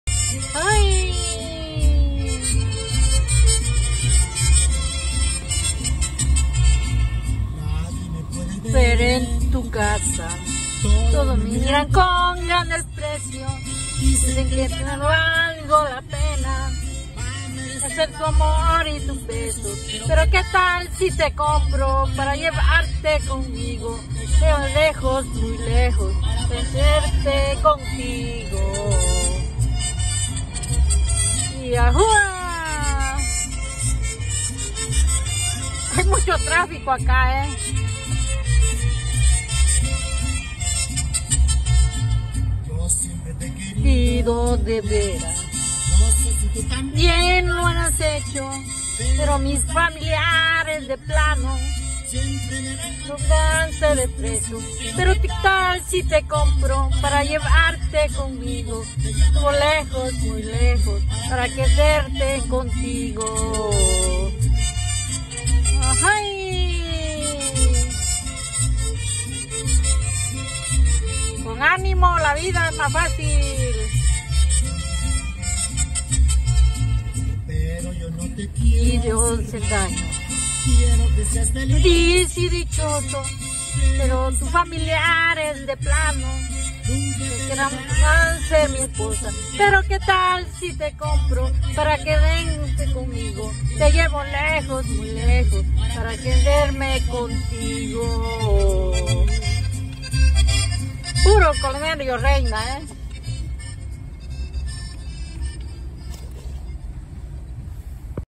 En el camino en San Francisco regreso a la casa pero cantando cover
Octubre/11/22